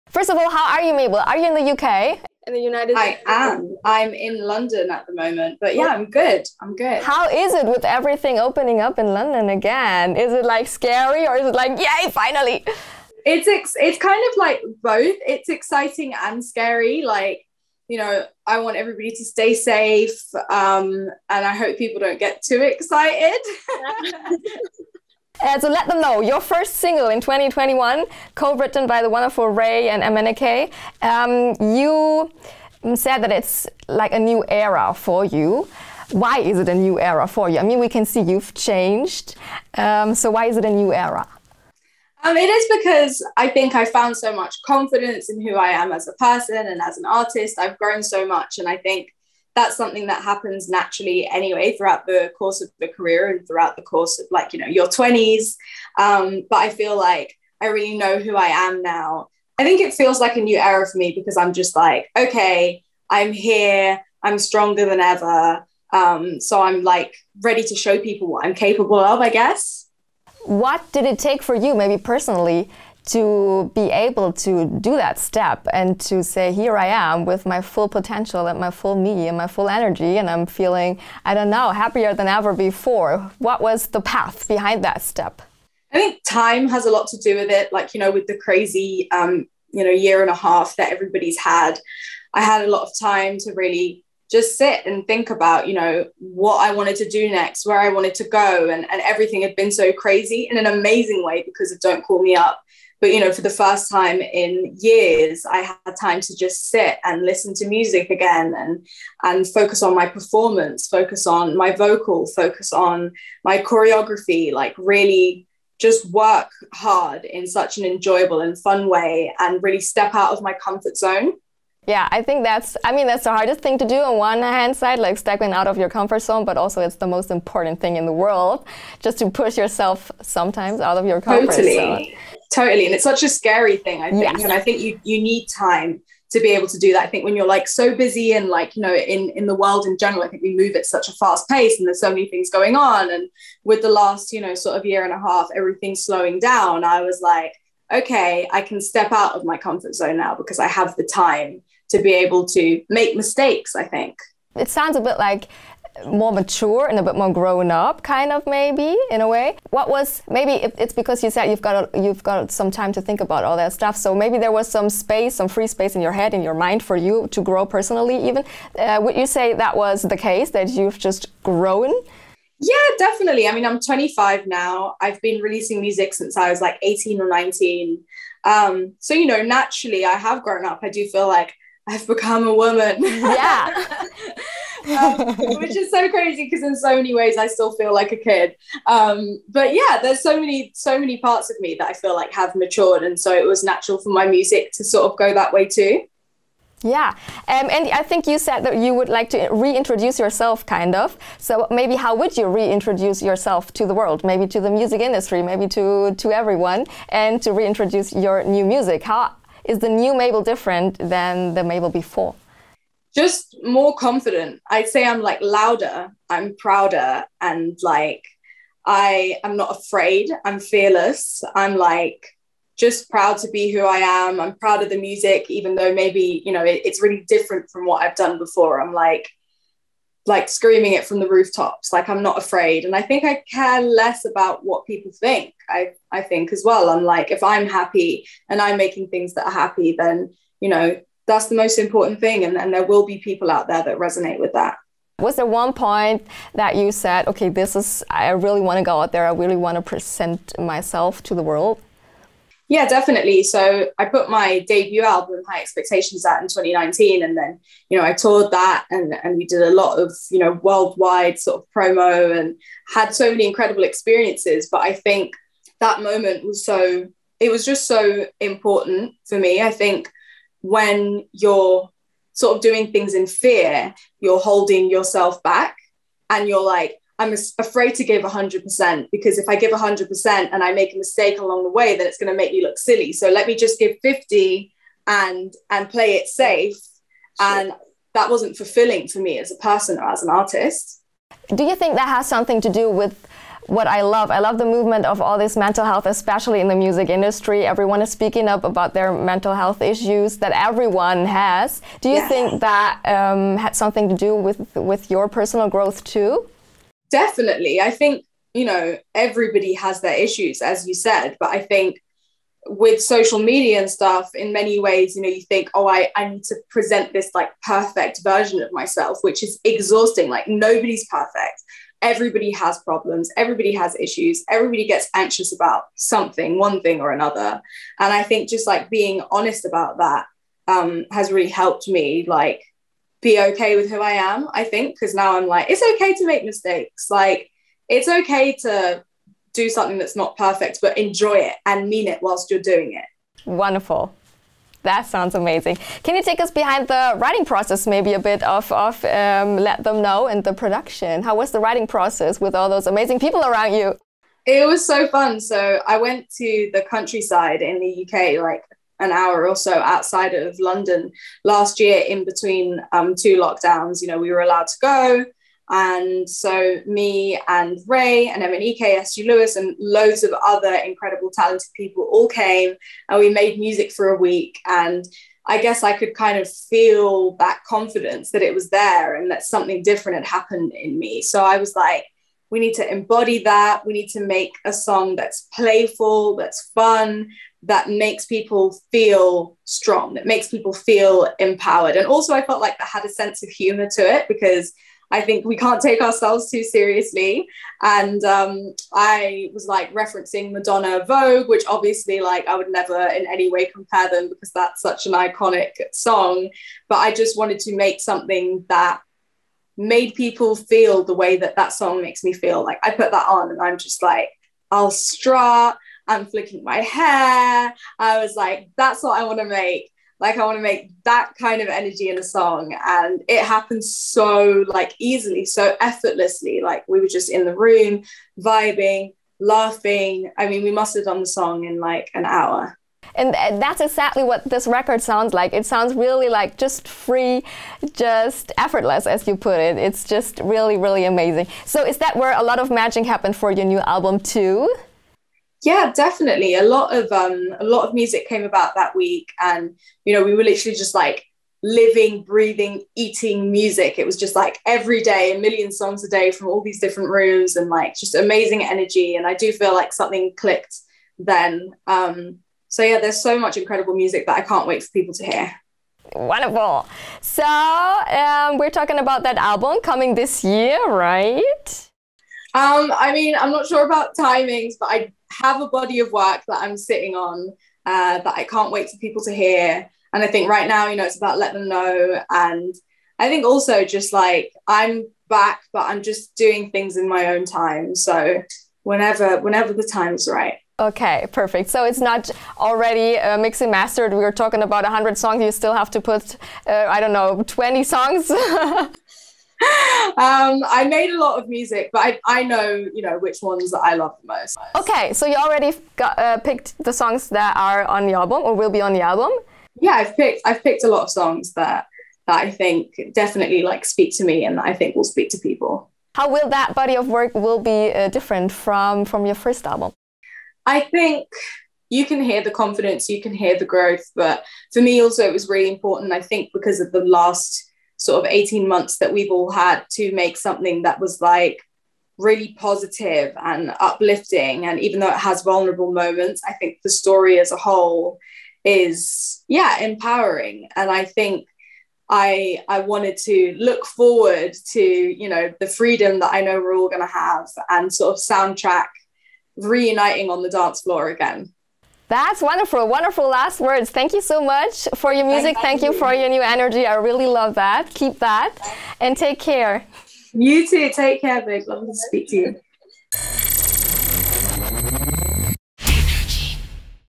Mabel On Her New Era: More Confident, Louder, Prouder, Fearless
Selbstbewusster, lauter, stolzer und furchtlos - das ist die "neue" Mabel, wie sie selbst sagt. Die Sängerin hat Mona im ENERGY Interview verraten, was ihre neue Ära ausmacht.

►ENERGY
Mehr Infos gibt's immer auf energy.de!